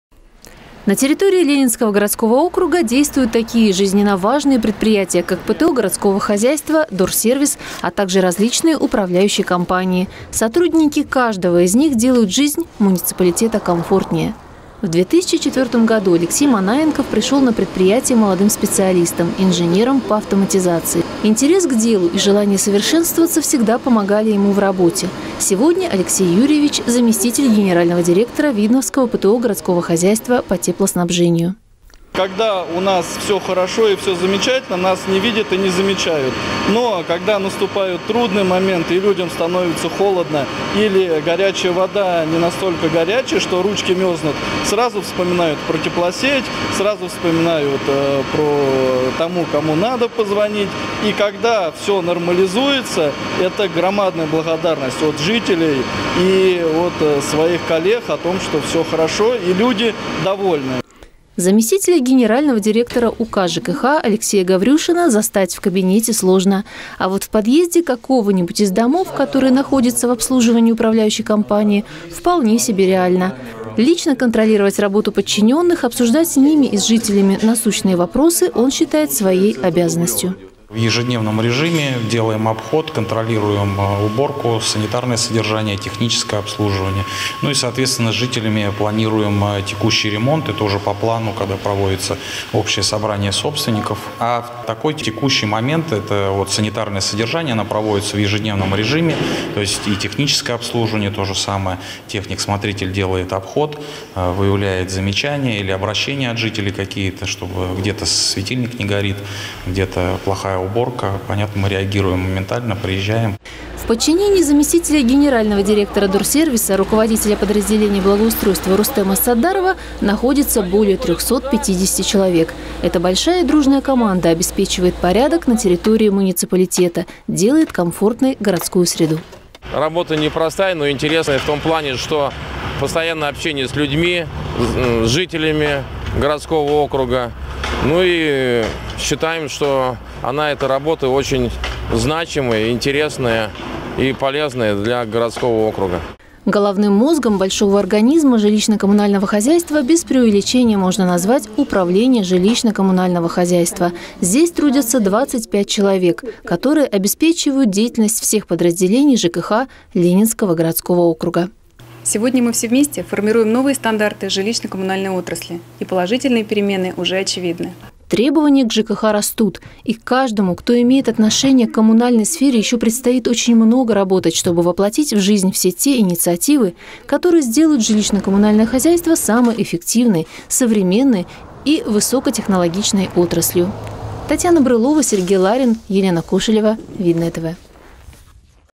НА ПЕРЕДОВОЙ
День работников ЖКХ ежегодно отмечается в третье воскресенье марта. Праздник призван обратить внимание на то, какую важную роль в нашей жизни играет структура коммунального обслуживания и её сотрудники. Герои нашего сюжета находятся на передовой, обеспечивая жителям муниципалитета комфортную жизнь в домах, во дворах, в городе Видное и других больших и малых населенных пунктах округа.